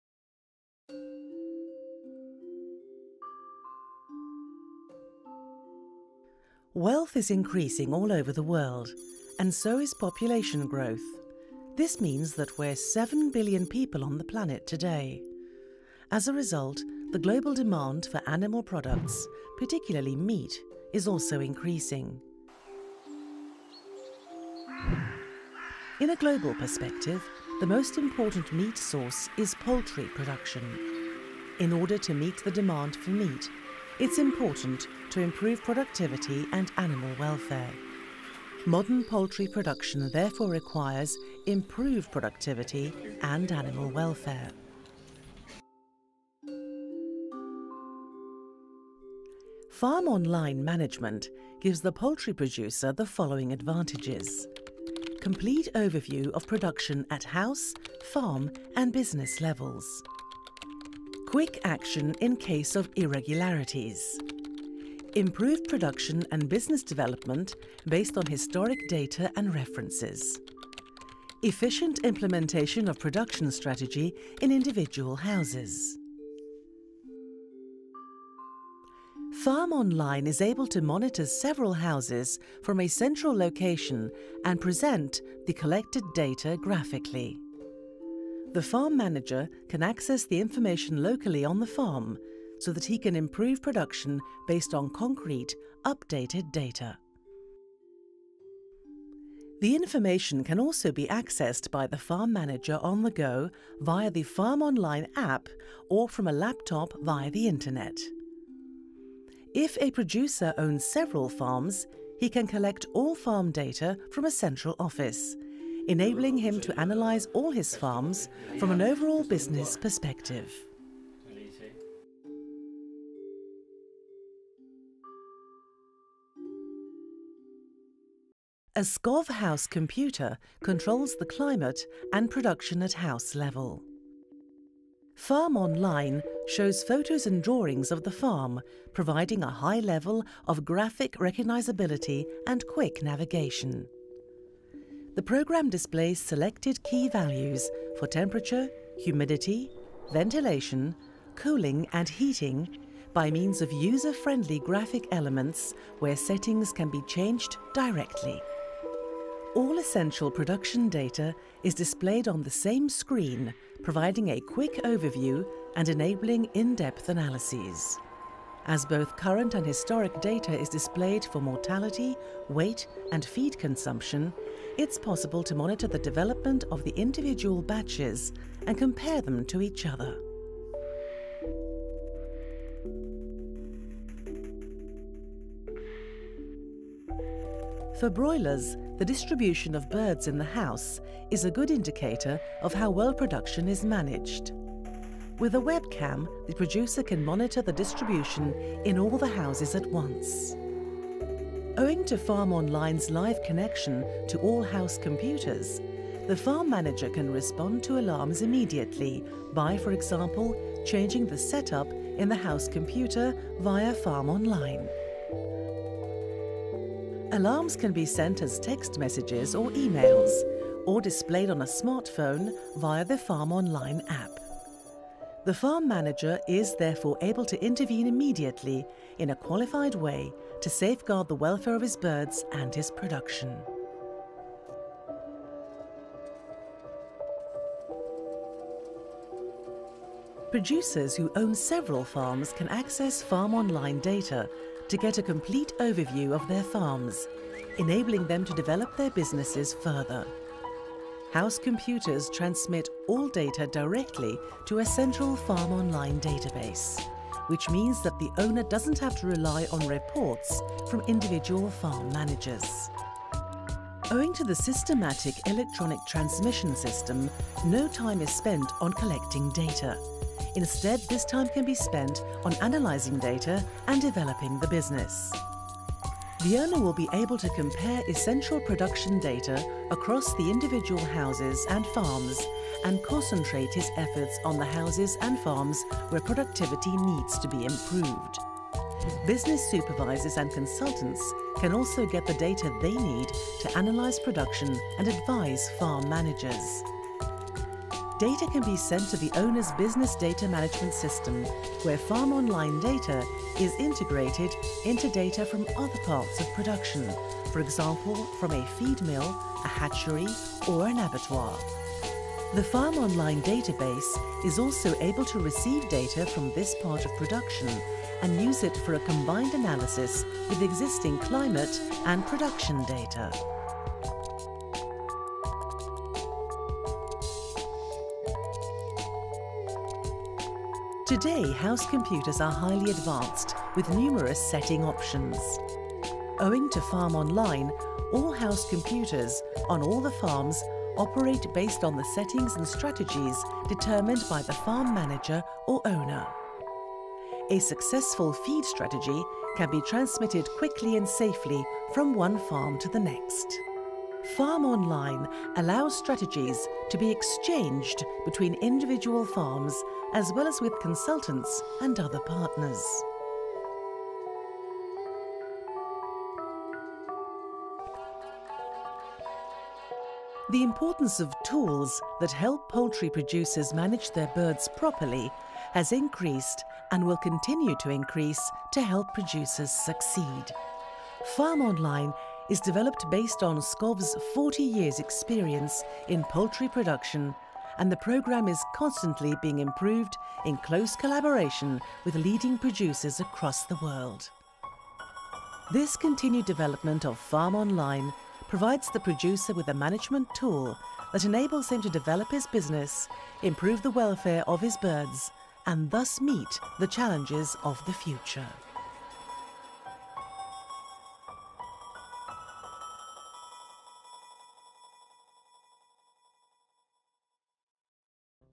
SKOV FarmOnline Poultry Chinese version
FarmOnline® is a management tool that SKOV has developed for use by the ever-larger pig and poultry farms. The system ensures the producer the highest possible yield from his production facility.